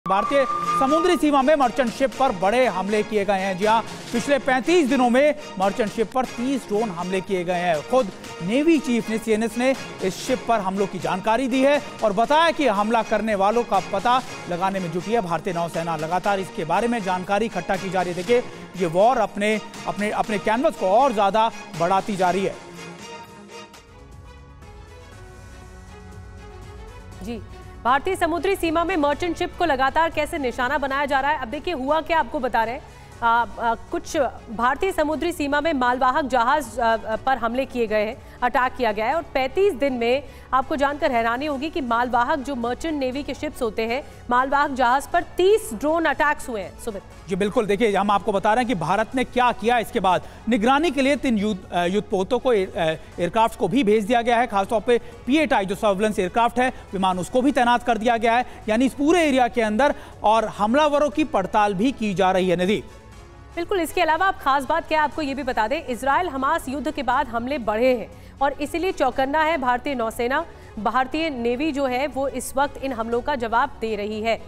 Navy Chief का बड़ा खुलासा, 35 दिनों में मर्चेंट शिप पर हुए 30 Drone हमले | Breaking News | TV9 Hindi
भारतीय समुद्री सीमा में मर्चेंट शिप पर हमले बढ़े. पिछले 35 दिनों में मर्चेंट शिप पर 30 ड्रोन हमले हुए.